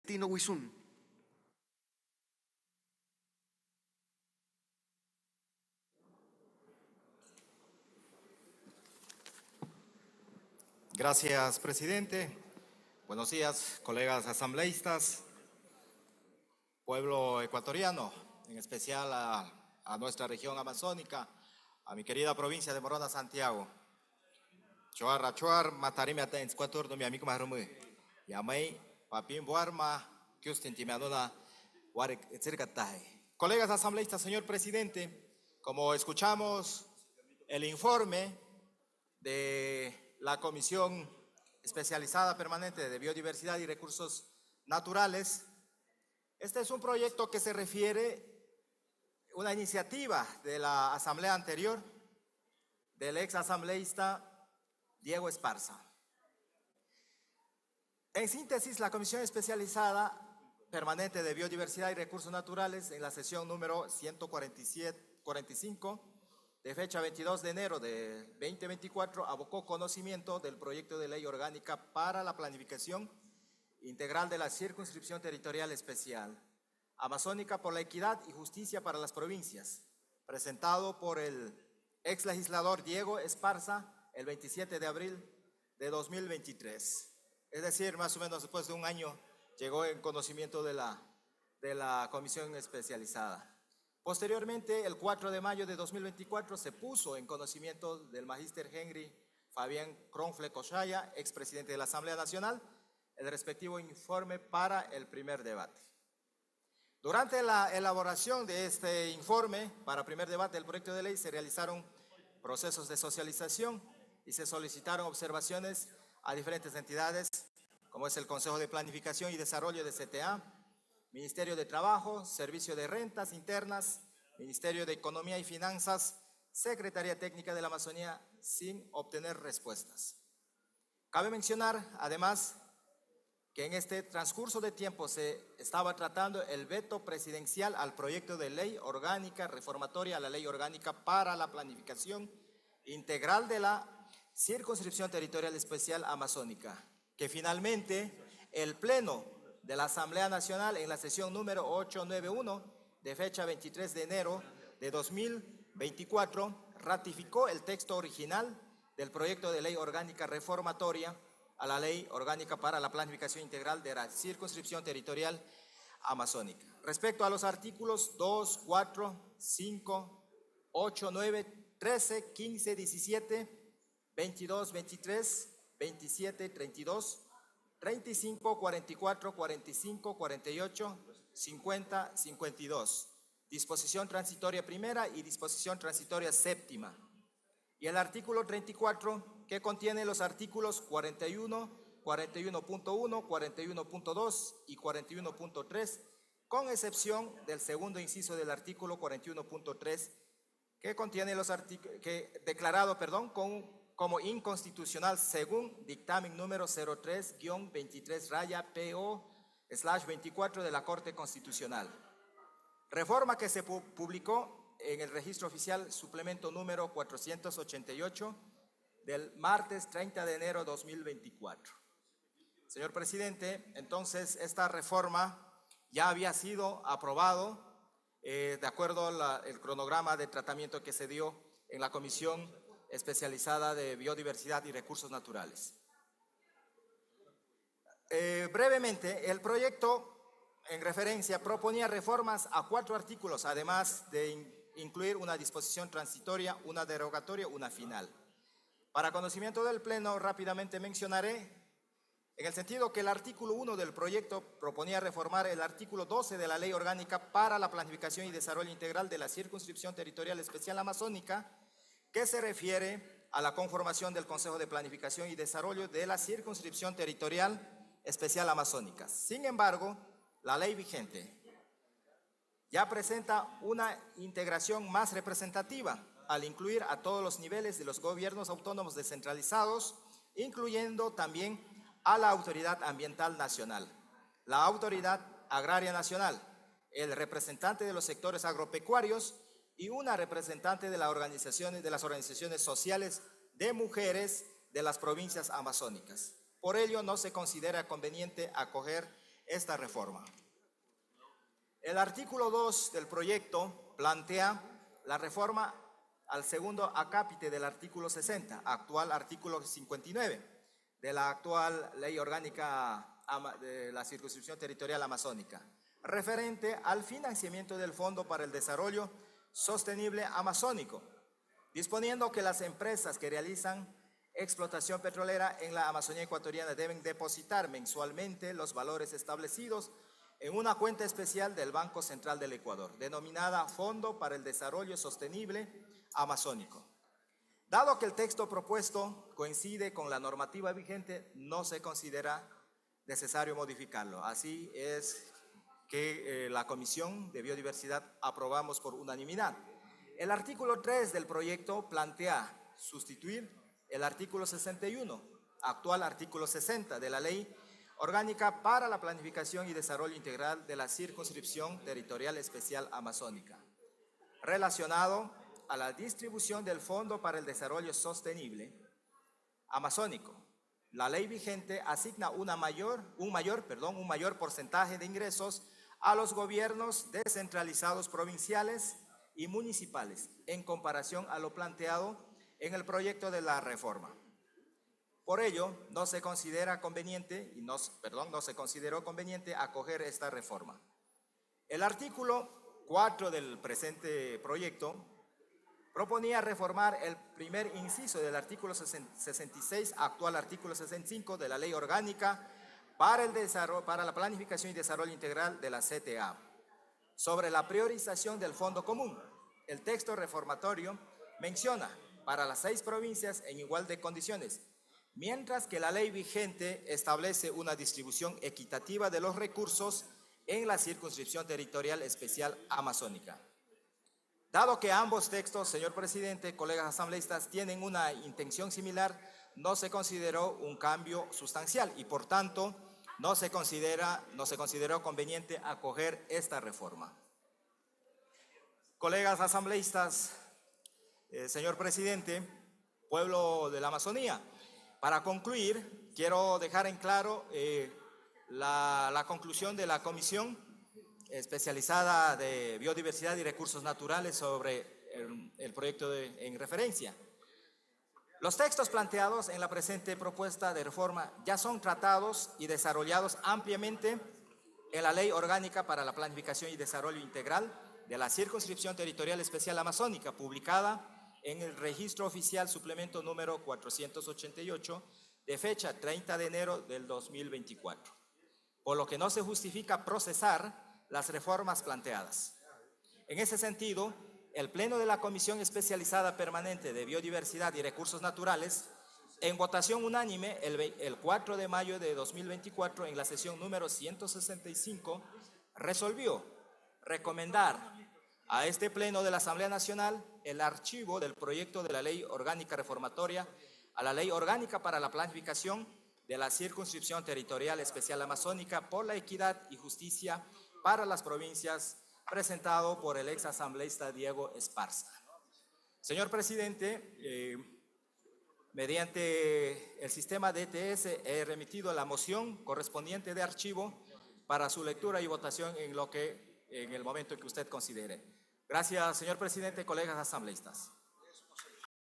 Asambleísta Celestino Wisum - Sesión 988
11 de diciembre de 2024
Informe para Primer Debate del Proyecto de Ley Orgánica Reformatoria a la Ley Orgánica para la Planificación de la Circunscripción Territorial Especial Amazónica por la Equidad y Justicia para las Provincias.